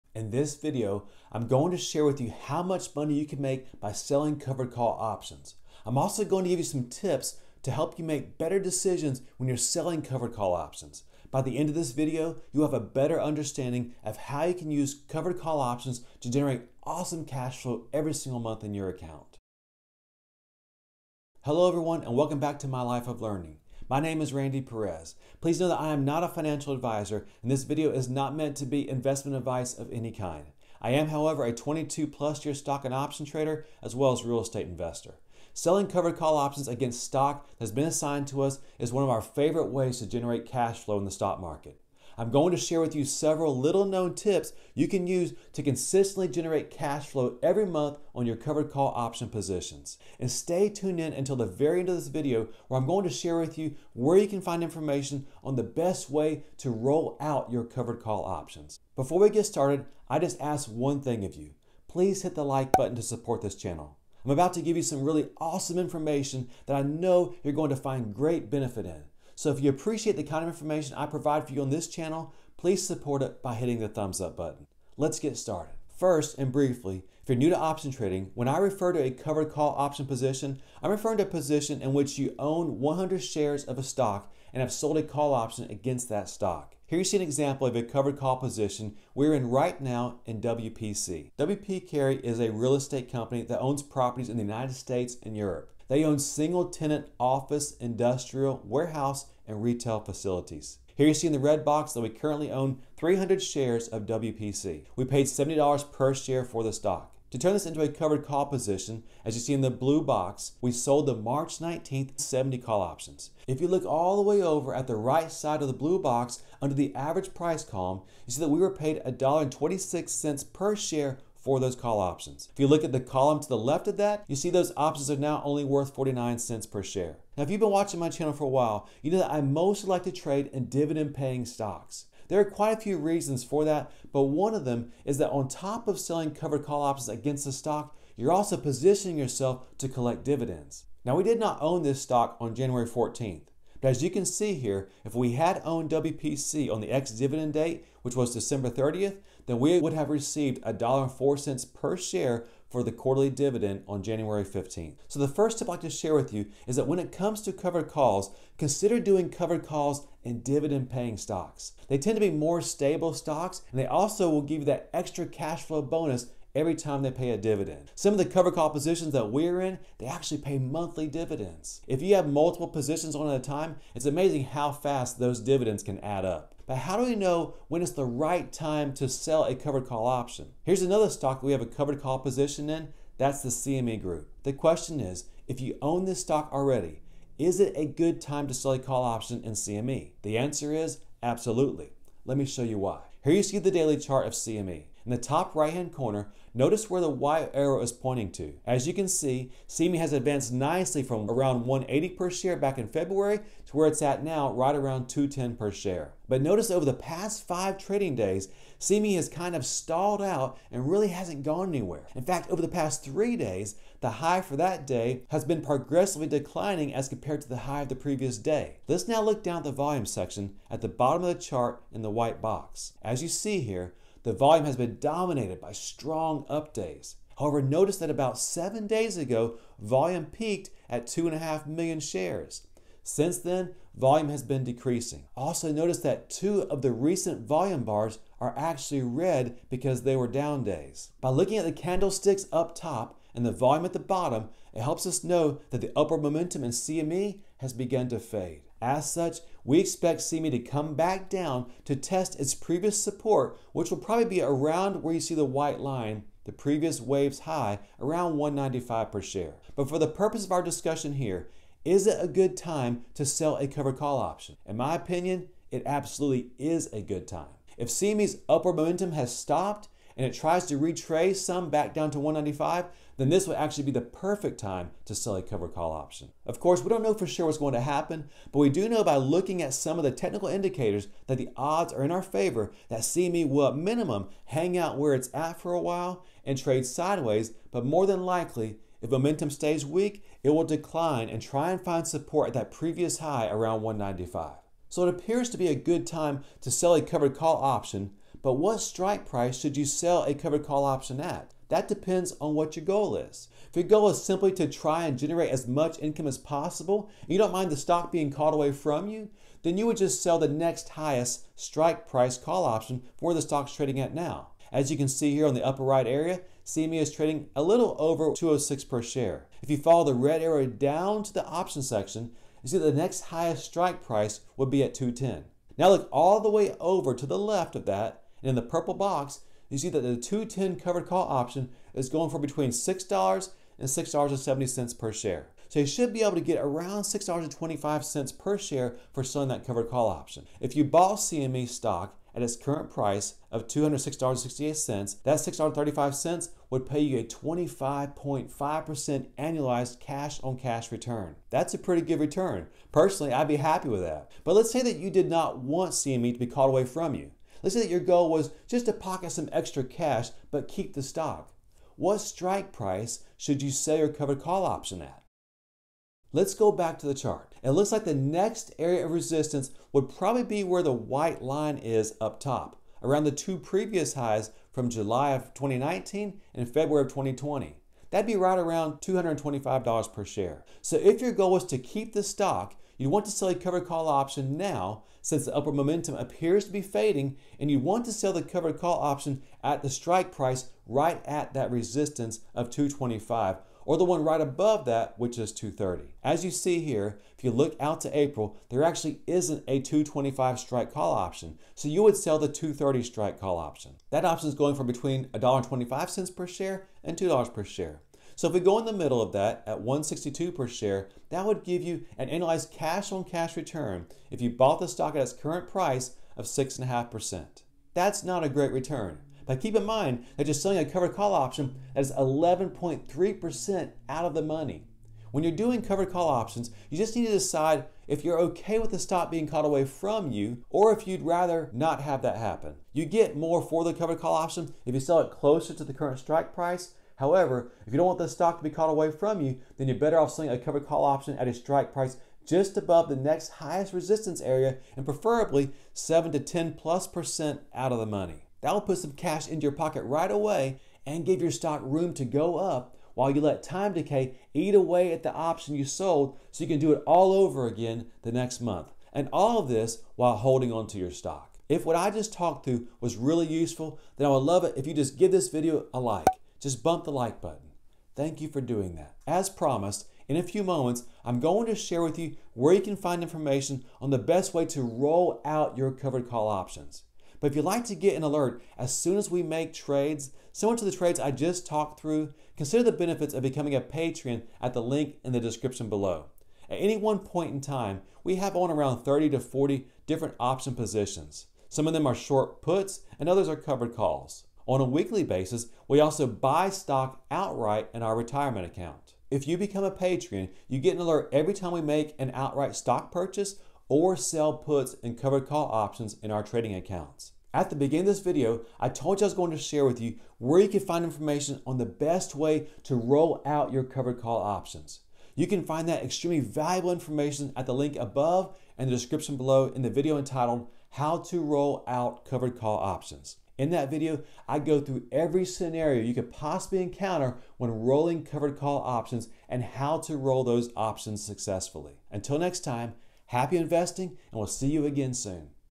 Selling Covered Calls Tips - How much Money can I Make Selling Covered Calls for Monthly Income
📕 Get a copy of my My Option Trading eBook "New Beginnings: The Option Trading Story"

⏰Chapters ⏰
0:00 Intro
1:15 Covered calls explained simply (Example WPC)
2:06 Selling Covered Call Tip #1 - Sell covered call options on dividend stocks
2:56 Selling Covered Call Tip #2 - When is the best time to sell a covered call option
4:57 Selling Covered Call Tip #3 - What strike price should you sell a covered call option at
5:14 How much can you make selling covered calls

📕 Get a copy of my eBook "New Beginnings: The Option Trading Story"

In this video I’m going to share with you how much money you can make selling covered call options. I’m also going to give you some tips to help you make better decisions when you’re selling covered call options. By the end of this video you will have a better understanding of how you can use covered call options to generate awesome cash flow every single month!

Selling covered call options against stock that has been assigned to us is one of our favorite ways to generate cash flow in the stock market. I’m going to share with you several little-known tips you can use to consistently generate cash flow every month on your covered call option positions.

Here you see an example of a covered call position we are in right now in WPC. We paid $70 per share for the stock. To turn this into a covered call, we sold the March 19th $70 call options. We were paid $1.26 per share for those call options. If you look at the column to the left of that, you see those options are now only worth $0.49 per share. The first tip I’d like to share with you when it comes to covered calls is, consider doing covered calls in dividend paying stocks.

But how do we know when it’s the right time to sell a covered call option? Here is another stock that we have a covered call position in, that’s the CME group. The question is, if you owned this stock already is it a good time to sell a call option in CME? The answer is absolutely! Let me show you why.

So you should be able to get around $6.35 per share for selling that covered call option. If you bought CME stock at its current price of $206.68, that $6.35 would pay you a 25.5% annualized cash on cash return! That’s a pretty good return.

But let’s say that you did not want CME to be called away from you and your goal was to just pocket some extra cash but keep the stock,. What strike price should you sell your covered call option at? Let’s go back to the chart. It looks like the next area of resistance would probably be where the white line is up top, around the two previous highs from July 2019 and February 2020.

When you’re doing covered call options you just need to decide if you’re OK with the stock being called away from you or if you'd rather that not happen. You get more for the covered call if you sell it closer to the current stock price, however if you don’t want to sell the stock then you’re better off selling covered calls at a strike price just above the next highest resistance area and preferably 7% to 10+% out of the money.

Happy investing!
Randy Perez